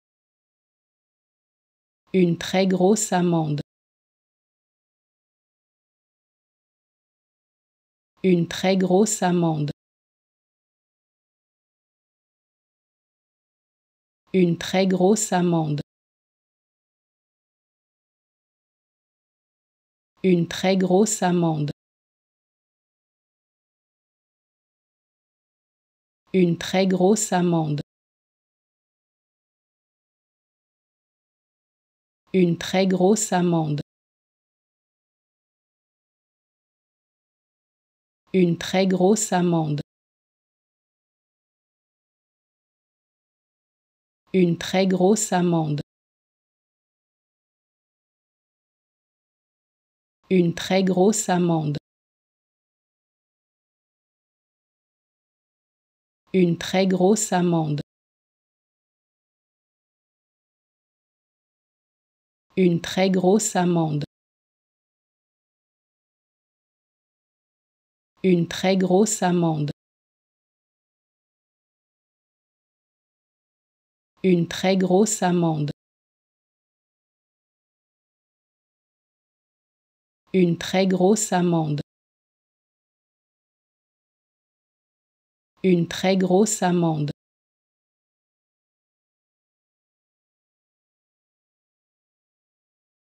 How to Pronounce ''Une très grosse amende'' (A very large fine) Correctly in French
Learn how to say and properly pronounce ''Une très grosse amende'' in French with this free pronunciation tutorial.